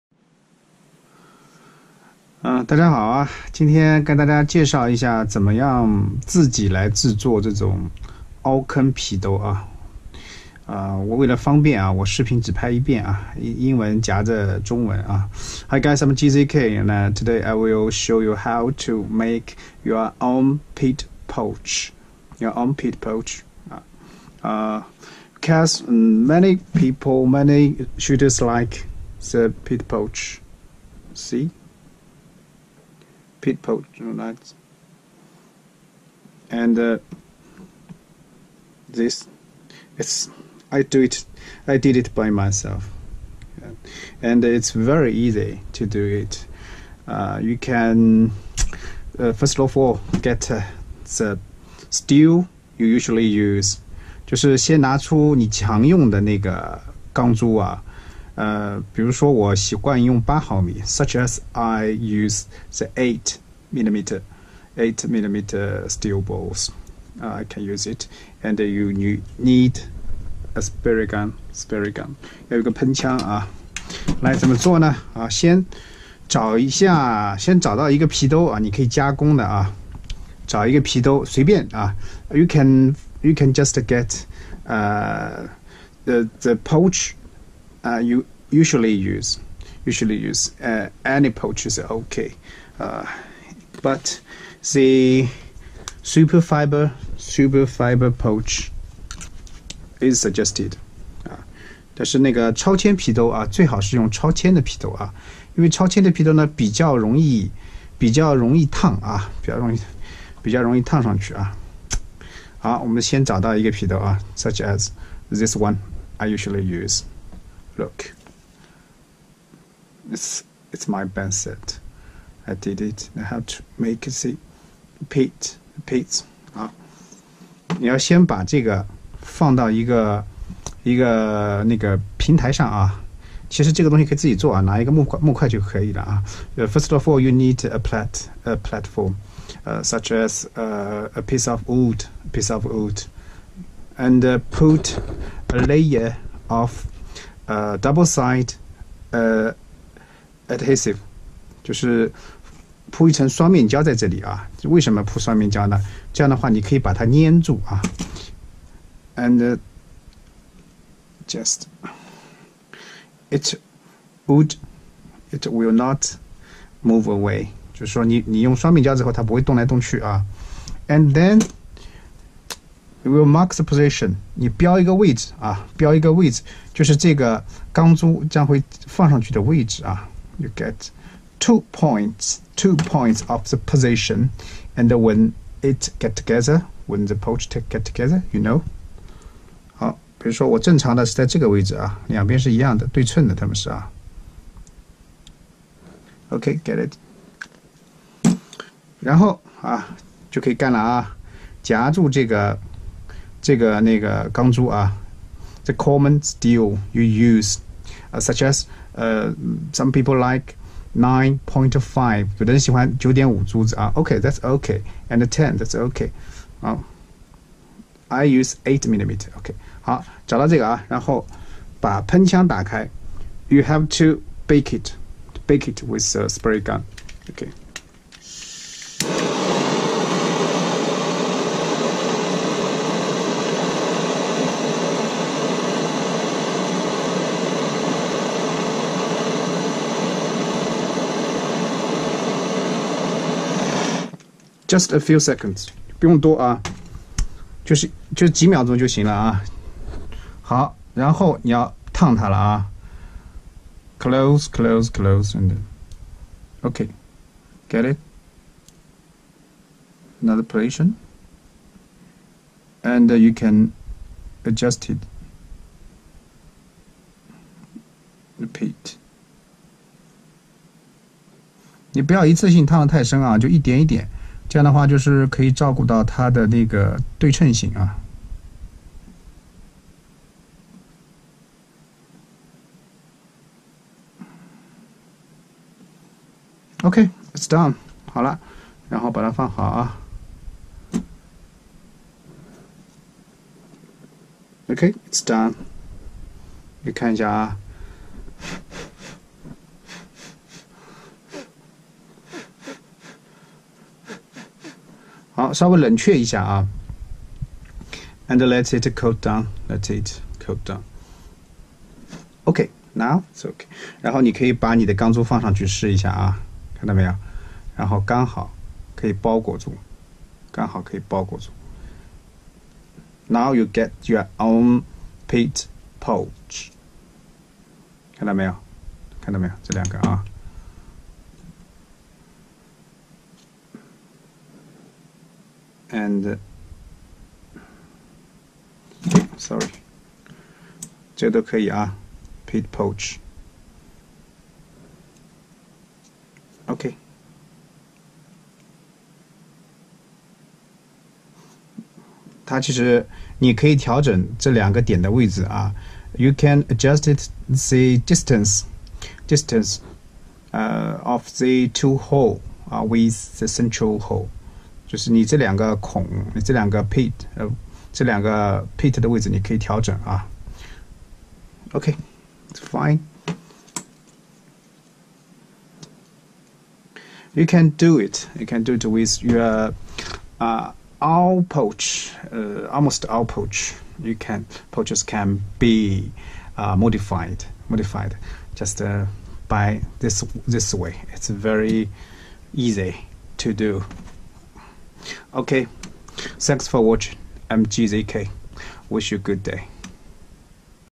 How to make your own Pit Locating Pouch? Simple enough!
If you like the Pit Locating Pouch, this video should help you. In fact, most of the pit pouch products are also produced in this way, but the operation is more standardized. For every shooter, custom or self-made accessories are the best for use, so let's do more DIY.